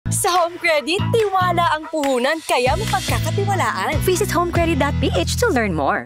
11 Million Customers
MARAMING SALAMAT SA TIWALA!

Happy 11 years, mga Ka-HC! Halos 11 million na Pilipino na ang pumili sa Home Credit at 15,000 partner stores na kasama nating magbigay saya sa buong Pilipinas! Salamat sa patuloy na tiwala kasama mo si Marian Rivera at Home Credit